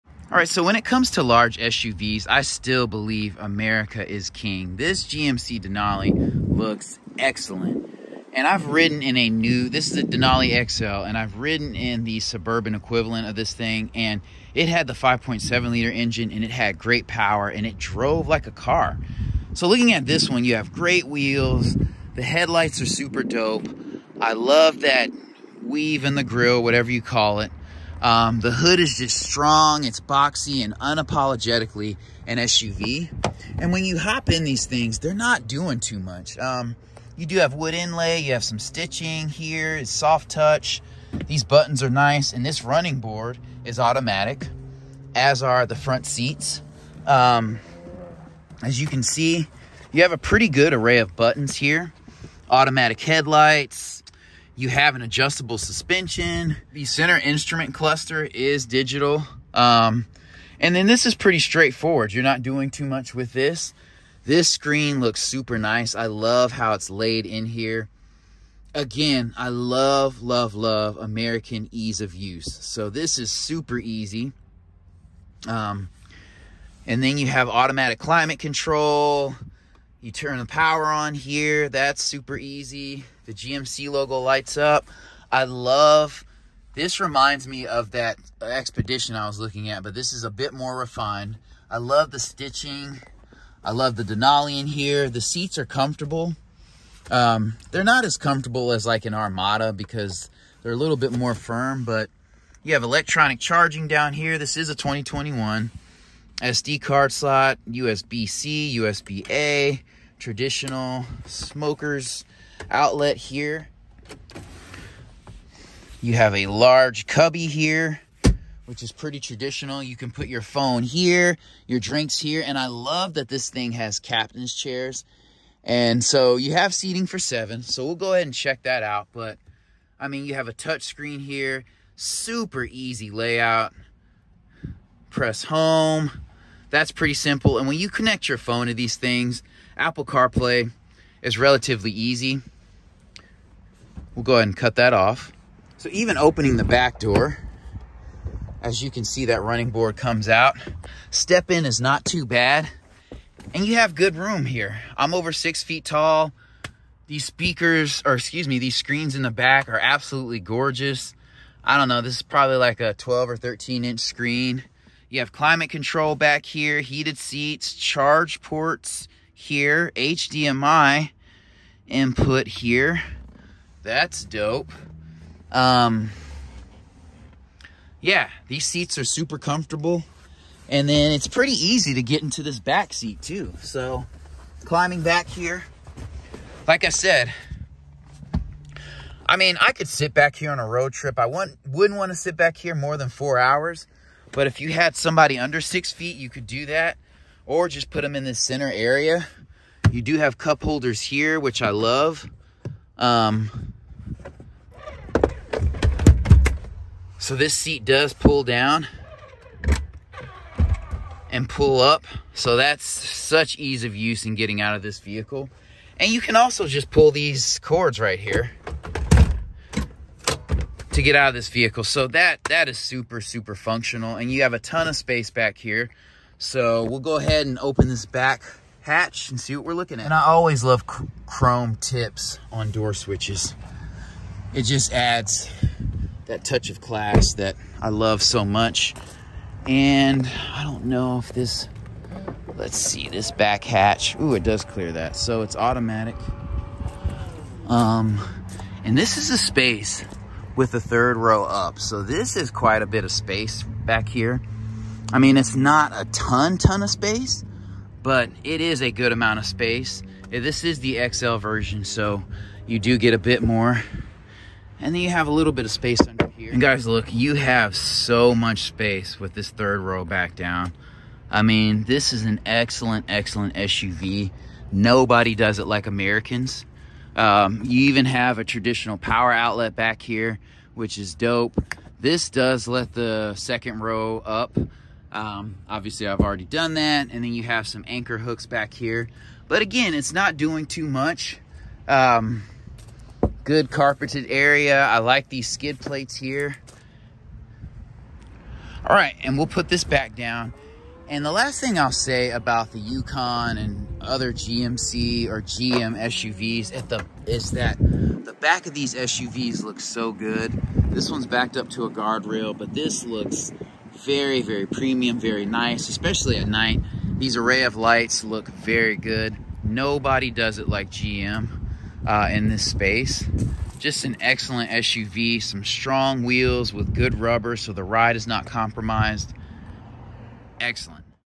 2021 GMC Denali…US still king of suvs
This Denali is a reminder of how great domestic SUVs are, and always have been.